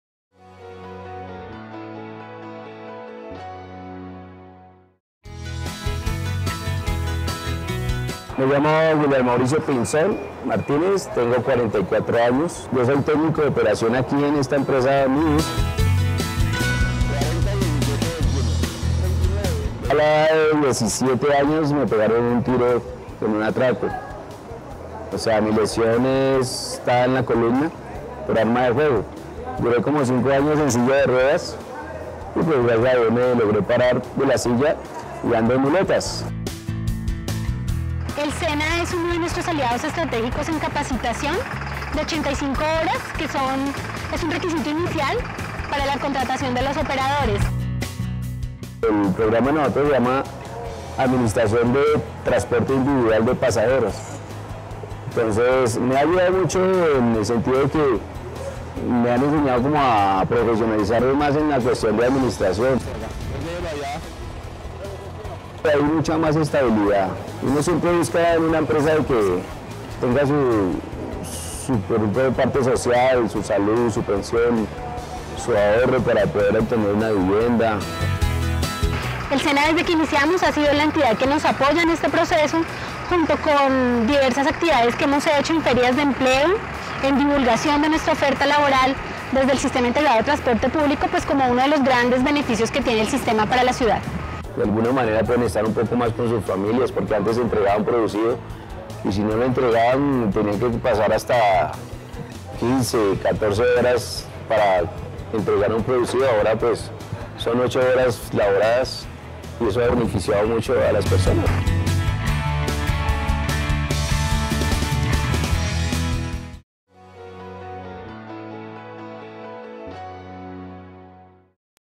Alianza del SENA y operadores del SITP beneficia a terceros afectados
El SENA, a través de la Agencia Pública de Empleo, participa en la creación de nuevas unidades productivas en el sistema de transporte público. Esto incluye formación para cualificar el perfil profesional